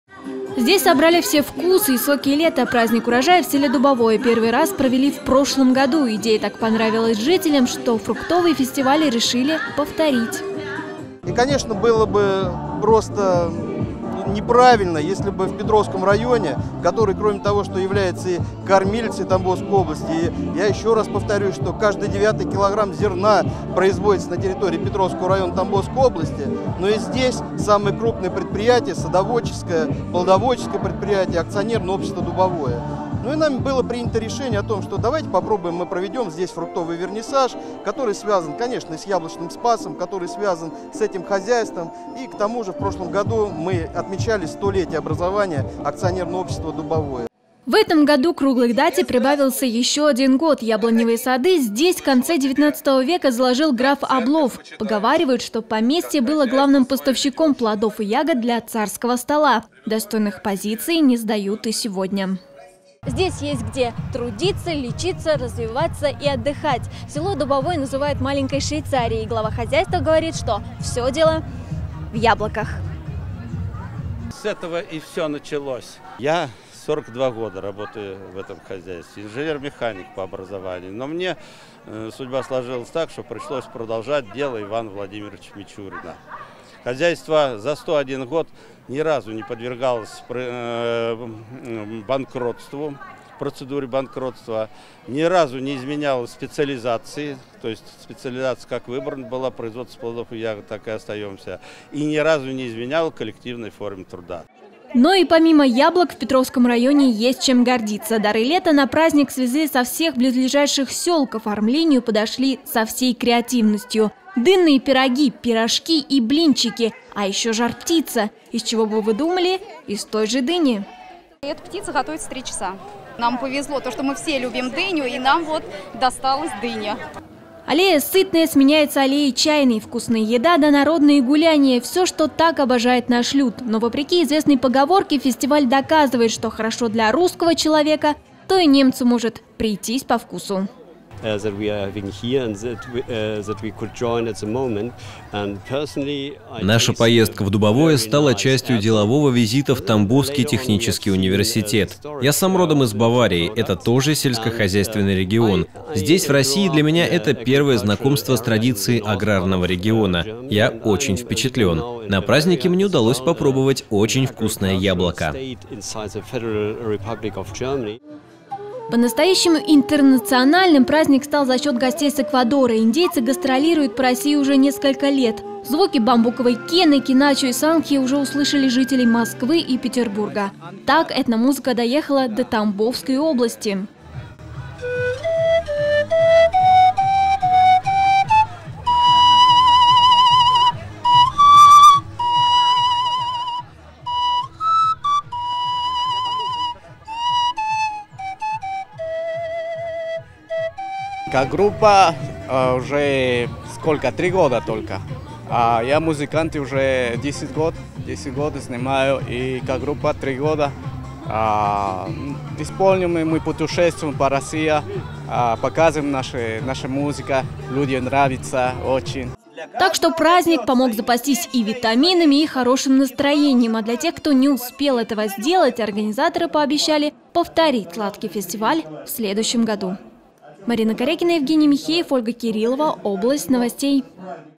На фестиваль «Фруктовый вернисаж» в Петровский район привезли фрукты и ягоды со всей области
Здесь собрали все вкусы и соки лета. Праздник урожая в селе Дубовое первый раз провели в прошлом году. Идея так понравилась жителям, что фруктовый фестиваль решили повторить.
Яблоневые сады здесь, в конце 19 века, заложил граф Облов. Поговаривают, что поместье было главным поставщиком плодов и ягод для царского стола. Достойных позиций не сдают и сегодня. Но и помимо яблок в Петровском районе есть  чем гордиться.  Дары лета на праздник свезли со всех близлежащих сел.